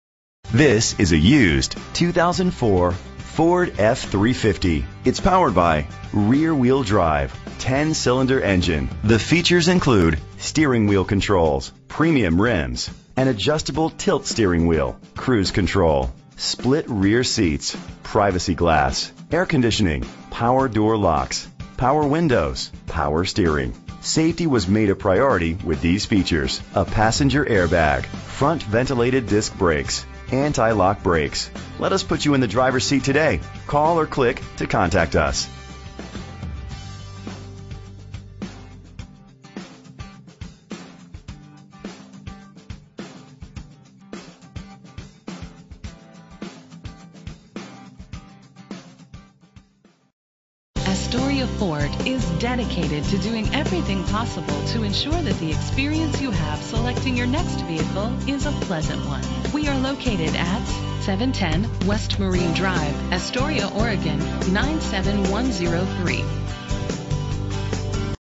2004 Ford F-350 - Warrenton OR
Year: 2004
Make: Ford
Model: F-350
Trim: XLT
Engine: 6.8 liter V10 20 valve
Transmission:
Color: White
Mileage: 182240
Address:
1809 SE Ensign Lane
Warrenton, OR 97103